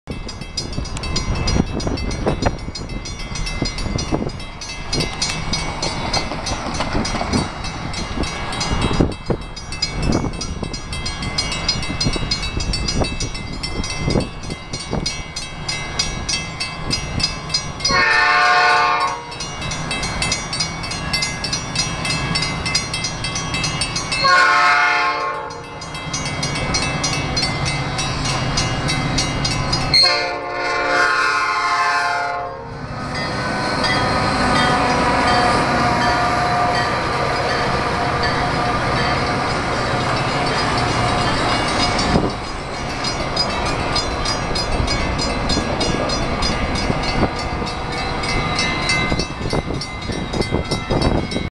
Outbound Metra train # 2225, an EMD F40PH # 122 goes through Galewood
Metra EMD F40PH engine # 122 heads west through Galewood on the Milwaukee District/West Line on Chicago's Northwest Side at Narragansett Avenue.